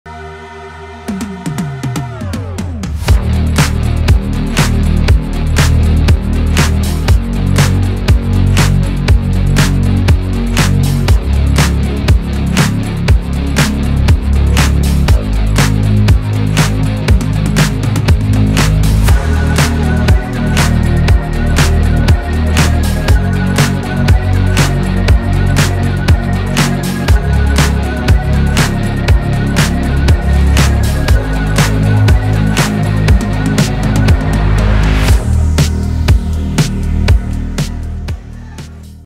BMW innovates with 3D campaign at Lisbon Airport | JCDecaux Portugal
Innovation never ends as the new BMW i5 takes on a new dimension at Humberto Delgado Airport, in an unprecedented campaign on JCDecaux media.